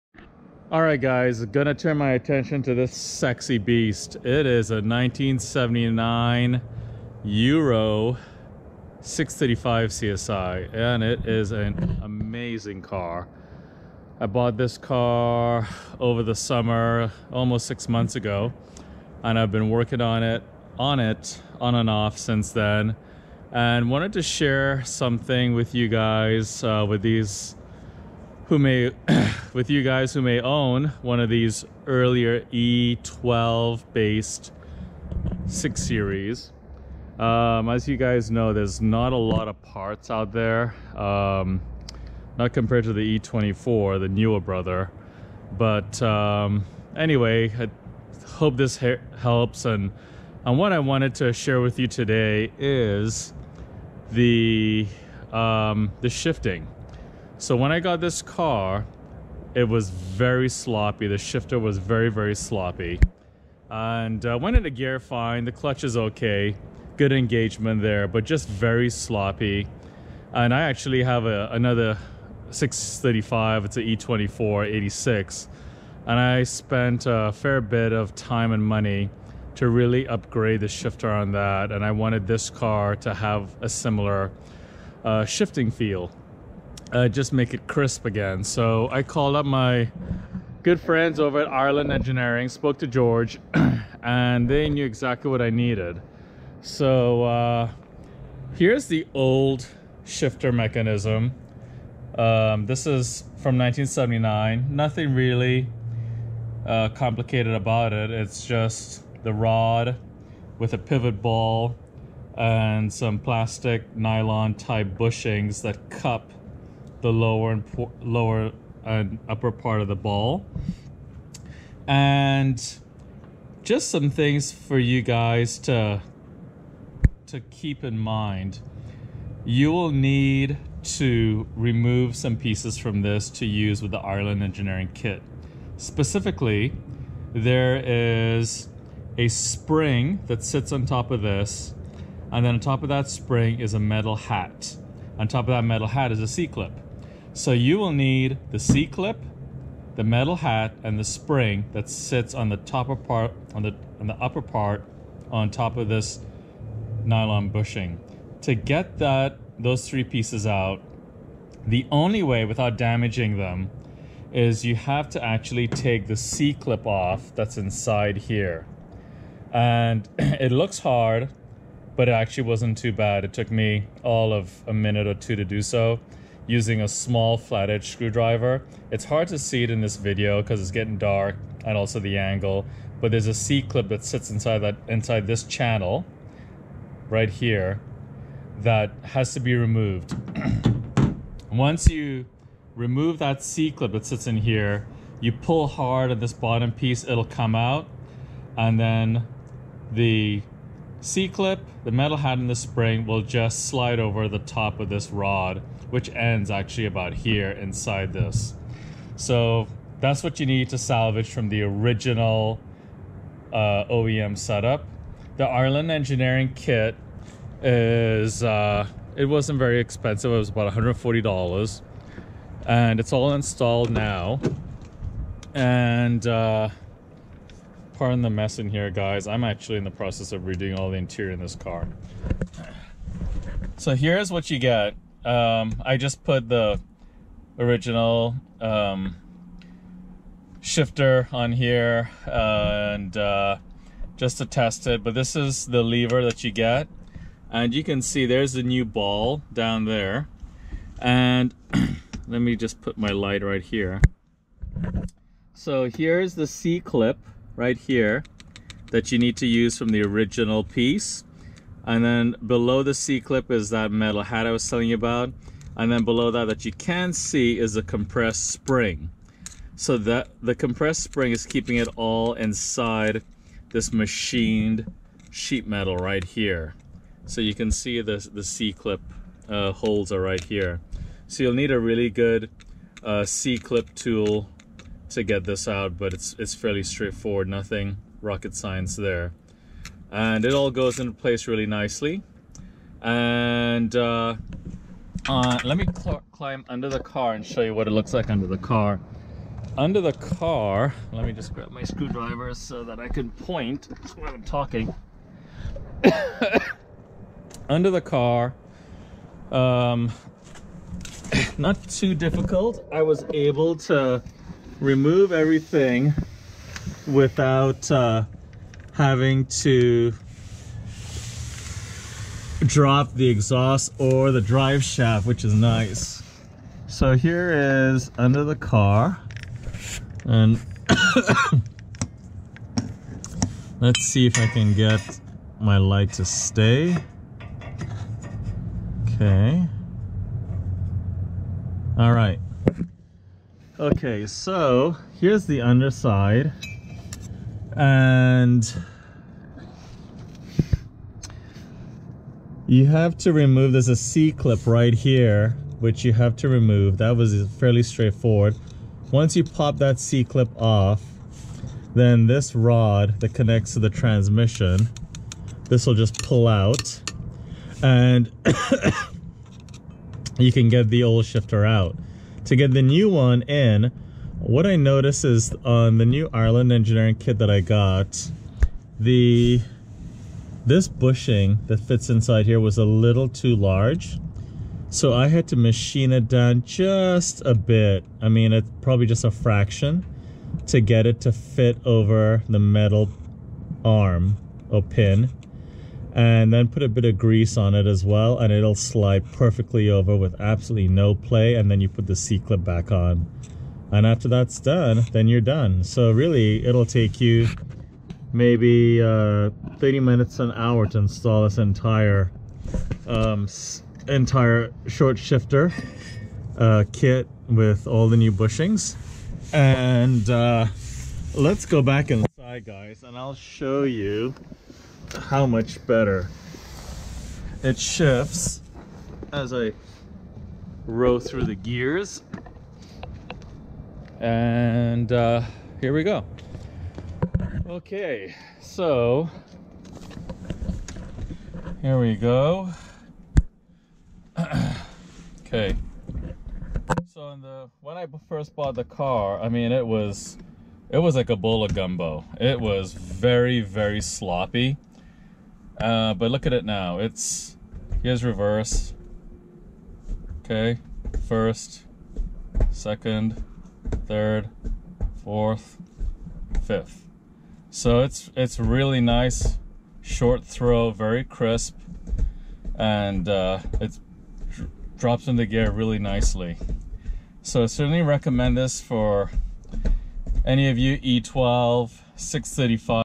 1979 BMW 635CSi E12 Ireland Engineering Short Shift Kit - In 30 Minutes
Installing Ireland Engineering's Short Shift Kit into an E12 based 635CSi.  Change those worn 40 year old bushings and improve the shift feel on your early CSi